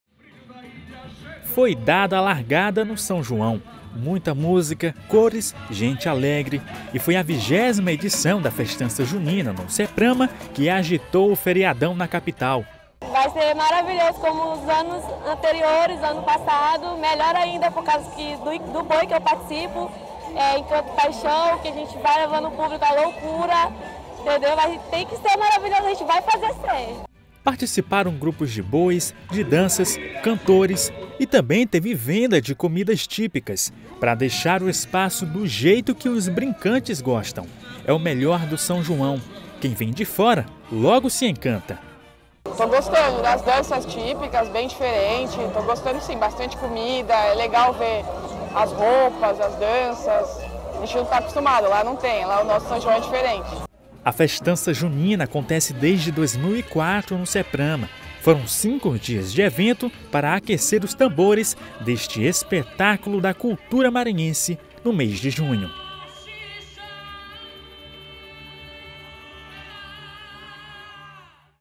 Começa o São João: festa no Ceprama agitou o feriadão em São Luís | JTV UFMA
📌 Acompanhe nossa programação pelos seguintes canais:
- Canal aberto: 16.1 digital
- NET-Claro: canal 17
- SKY: canal 316
- MAXX: canal 16